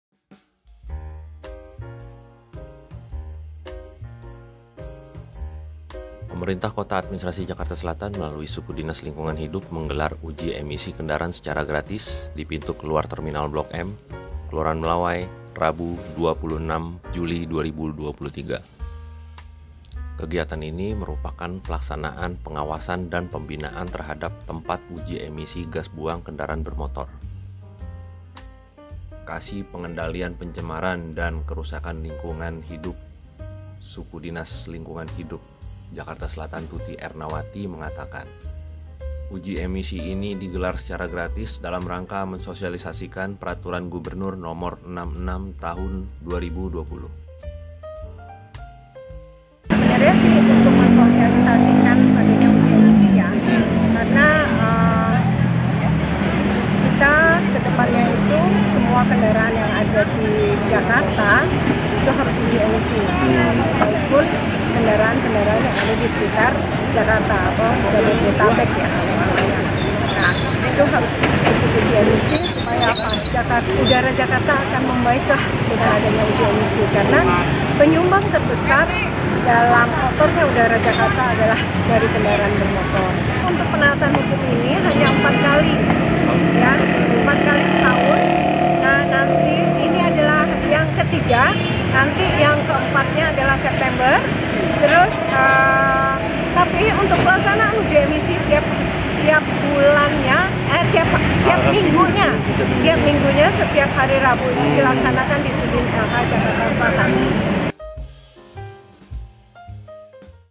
UJI EMISI GRATIS UNTUK PERBAIKAN KUALITAS UDARA JAKARTA RABU 26 JULI 2023
Pemerintah Kota Administrasi Jakarta Selatan melalui Suku Dinas Lingkungan Hidup menggelar Uji Emisi Kendaraan secara gratis di (Pintu Keluar Terminal Blok M) Jl. Sultan Hasanudin Kelurahan Melawai, Kecamatan Kebayoran Baru - Jakarta Selatan , Rabu (26/7).

Kegiatan ini merupakan pelaksanaan kegiatan Pengawasan dan Pembinaan terhadap tempat uji emisi gas buang kendaraan bermotor.

Kasi PKLH Tuty Ernawati mengatakan, uji Emisi penaatan hukum ini digelar secara gratis dalam rangka  mensosialisasikan peraturan Gubernur Nomor 66 Tahun 2020.

"Karena nantinya semua kendaraan yang ada di DKI Jakarta itu harus sudah teruji emisi, begitu juga kendaraan dari luar Jakarta yaitu Bodetabek, supaya udara Jakarta semakin baik dengan adanya uji emisi," katanya.

Pada pelaksanaan uji emisi ini, sambung Tuty, pihaknya bekerjasama dengan TNI, Polri, Satpol PP, Dishub dan instansi terkait lainnya.

"Uji emisi ini digelar hanya hari ini saja, namun tiap minggunya Sudin LH menggelar uji emisi di Halaman Kantor Lingkungan Hidup setiap hari rabu," tandasnya.

Pada pelaksanaan uji emisi ini  sebanyak 22 kendaraan roda empat berbahan bakar bensin, dan 18 kendaraan berbahan bakar untuk. Sementara itu, untuk ke daraan roda dua sebanyak 13 kendaraan.